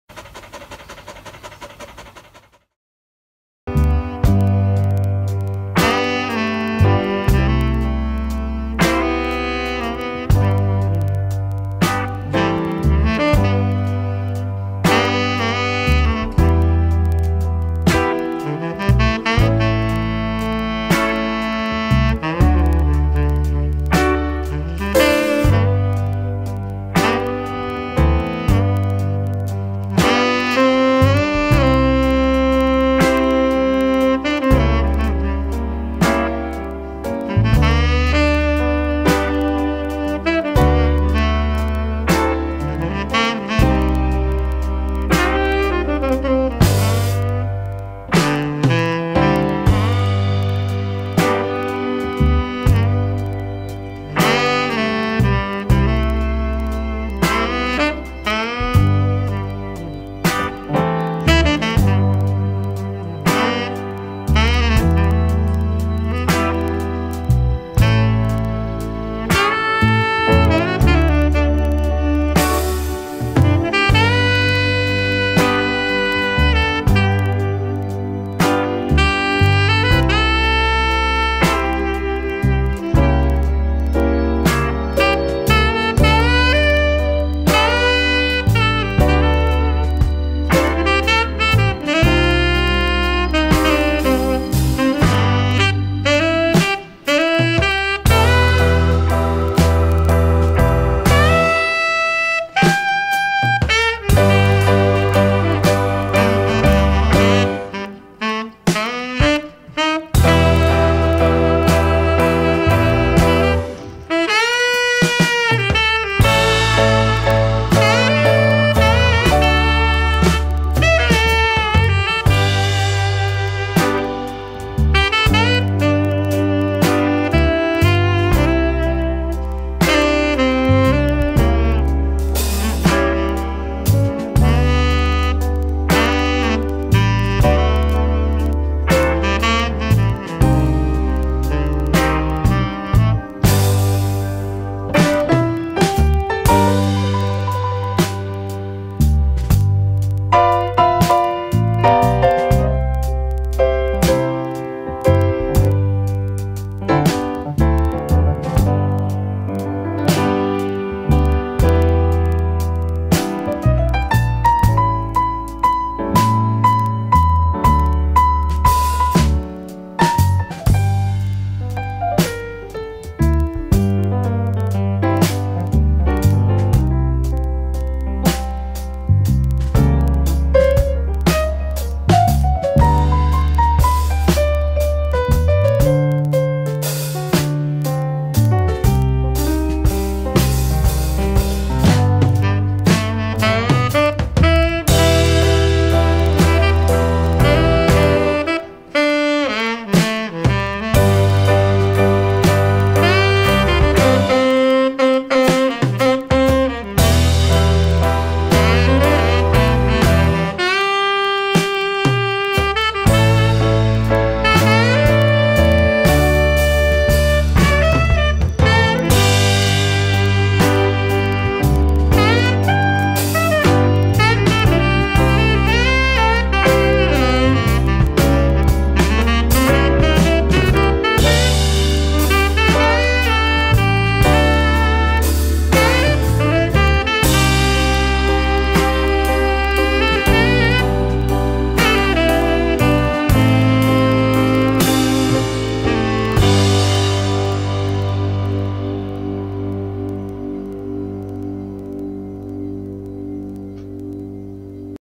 Landscape Photos of The Isle of Skye Scotland from a trip with Ben the Whippet
This video is about The Isle of Skye and photos of the area